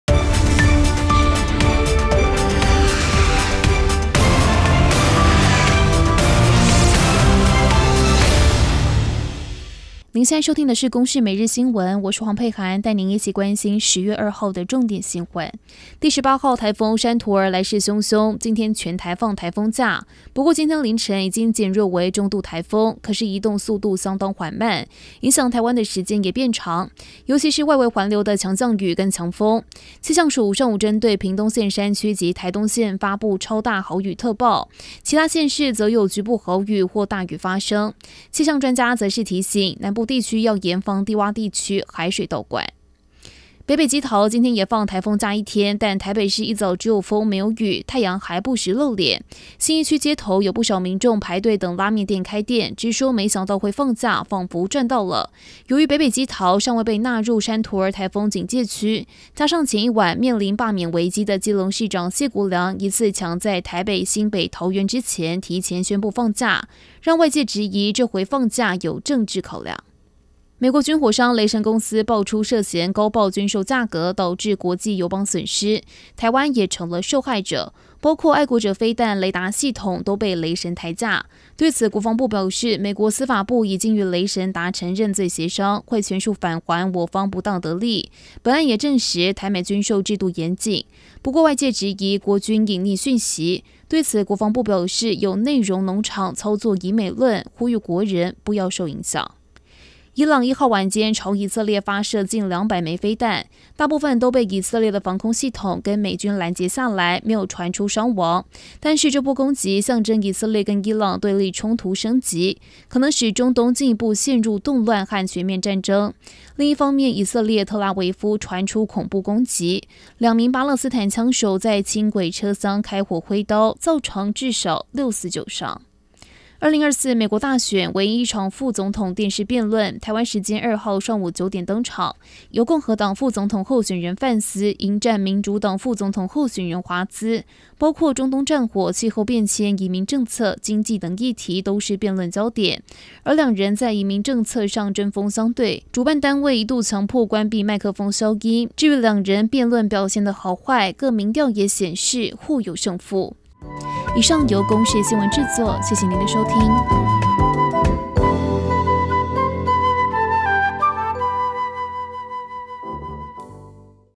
【新聞速報 Podcast】山陀兒移動緩慢影響延長 屏東山區、台東留意超大豪雨｜20241002公視新聞網
【新聞摘要】
第18號颱風”山陀兒”來勢洶洶，今天全台放颱風假，不過今天凌晨已經減弱為中度颱風，可是移動速度相當緩慢，影響台灣的時間也變長，尤其是外圍環流的強降雨跟強風，氣象署上午針對屏東縣山區及台東縣，發布「超大豪雨」特報，其他縣市則有局部豪雨或大雨發生，氣象專家則是提醒，南部地區要嚴防低窪地區、海水倒灌。

北北基桃今天也放颱風假一天，但台北市一早只有風、沒有雨，太陽還不時露臉。信義區街頭有不少民眾排隊等拉麵店開店，直說沒想到會放假，彷彿賺到了。由於北北基桃尚未被納入山陀兒颱風警戒區，加上前一晚，面臨罷免危機的、基隆市長謝國樑疑似搶在台北、新北、桃園之前，提早宣布放假，讓外界質疑這回放假有政治考量。

美國軍火商「雷神公司」爆出涉嫌”高報”軍售價格，導致國際友邦損失，台灣也成了受害者，包括愛國者飛彈、雷達系統，都被雷神抬價，對此，國防部表示，美國司法部已經與雷神達成認罪協商，會全數返還我方不當得利，本案也證實台美軍售制度嚴謹，不過外界質疑，國軍隱匿訊息，對此，國防部表示，有內容農場操作「疑美論」，呼籲國人不要受影響。

伊朗1號晚間，朝以色列發射近200枚飛彈，大部分都被以色列的防空系統跟美軍攔截下來，沒有傳出傷亡。但是這波攻擊，象徵以色列跟伊朗對立衝突升級，可能使中東進一步陷入動亂和全面戰爭。另一方面，以色列”特拉維夫”傳出恐怖攻擊，兩名巴勒斯坦槍手在輕軌車廂開火揮刀，造成至少6死9傷。

2024美國大選，唯一一場副總統電視辯論，台灣時間2號上午9點登場，由共和黨副總統候選人范斯，迎戰民主黨副總統候選人華茲，包括中東戰火，氣候變遷，移民政策，經濟等議題都是辯論焦點，而兩人在移民政策上針鋒相對，主辦單位一度強迫關閉麥克風消音。至於，兩人辯論表現的好壞，各民調也顯示互有勝負。

更多新聞內容，請鎖定今晚七點的公視晚間新聞，公視新聞是您全家人的新聞